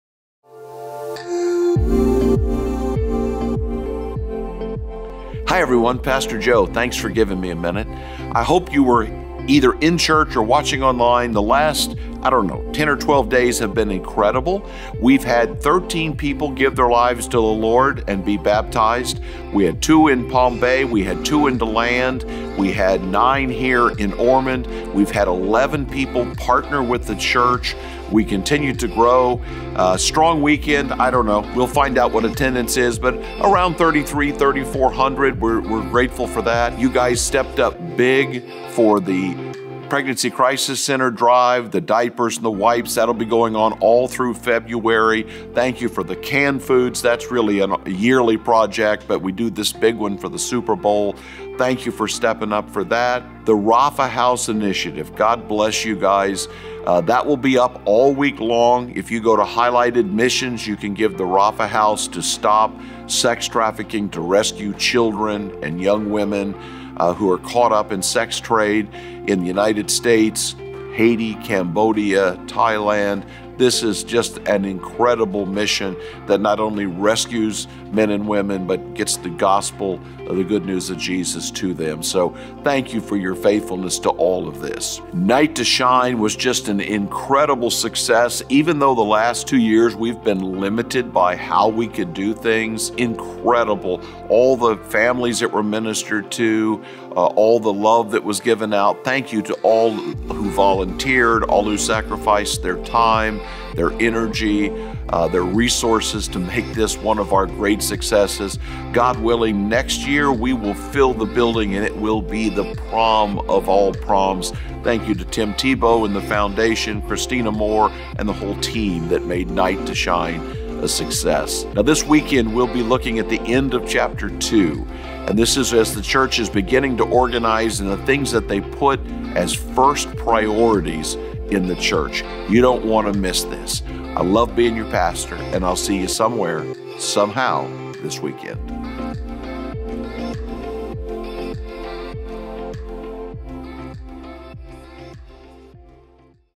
Pastor's Weekly Video - Feb. 16
This week, Pastor Joe celebrates baptisms, talks about the Souper Bowl Food Drive, Rapha House, Night to Shine and more.